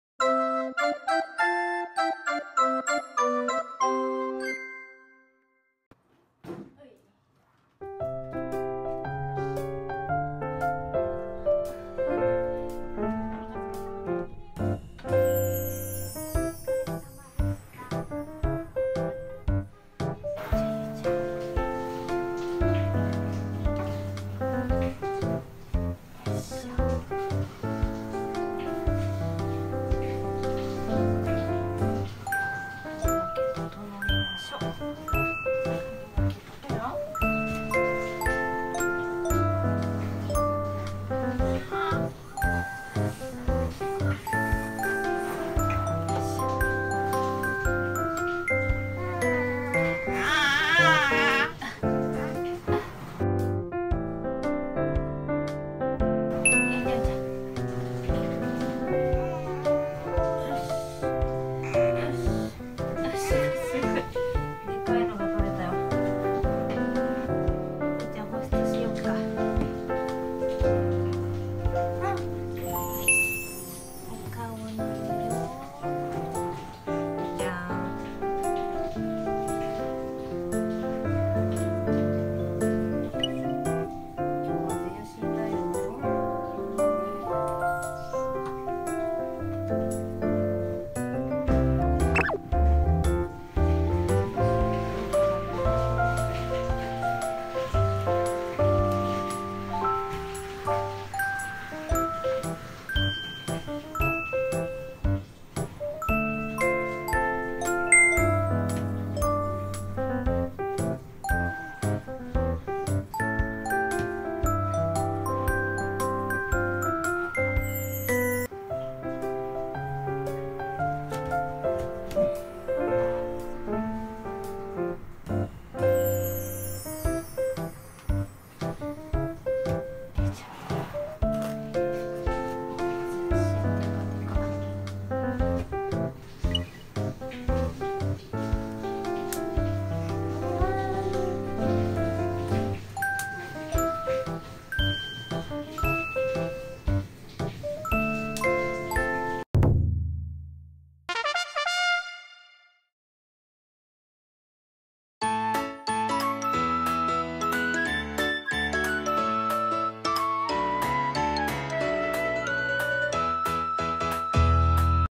【鼻吸い】赤ちゃんのお風呂上がりのスキンケア（プロペト、アトピタ使用）
赤ちゃんは保湿が大事っていうけど、うちの子は保湿すると赤くなることがあるので、季節や天気で塗ったり塗らなかったり試行錯誤です。